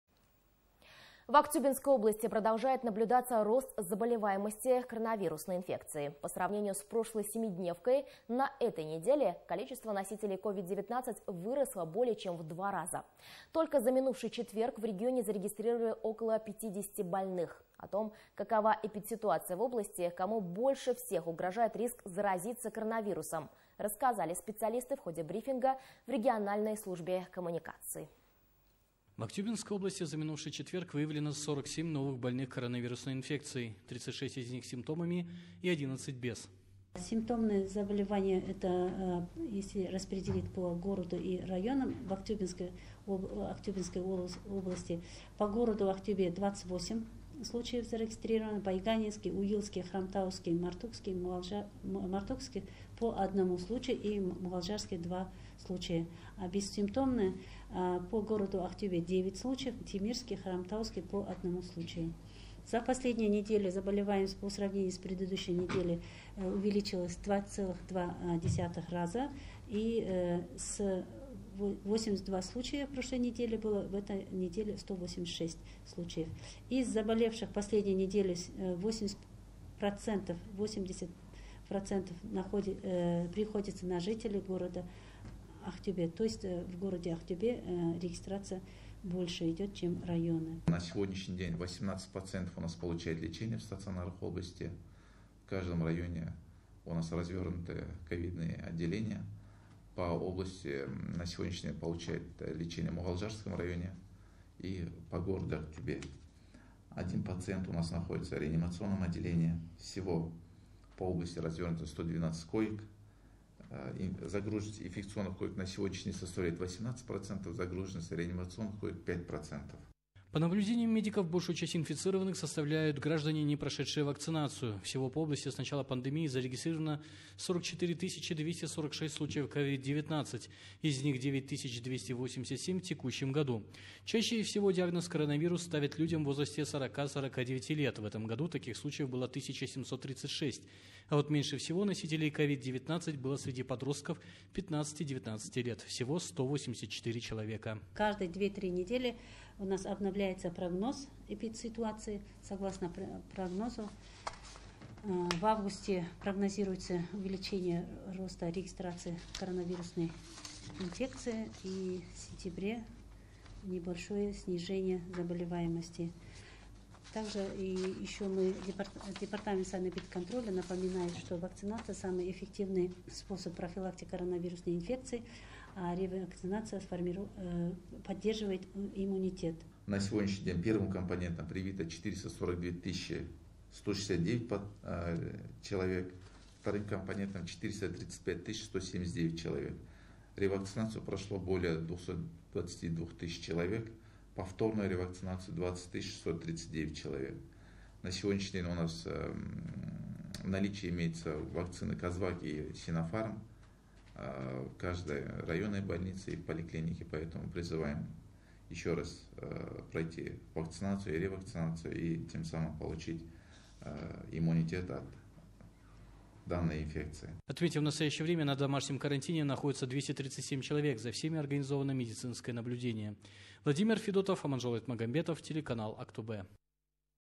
В ОБЛАСТИ COVID - 19 ЗАБОЛЕЛИ 47 ЧЕЛОВЕК
В Актюбинской области продолжает наблюдаться рост заболеваемости коронавирусной инфекцией. По сравнению с прошлой семидневкой, на этой неделе количество носителей Ковид-19 выросло более чем в два раза.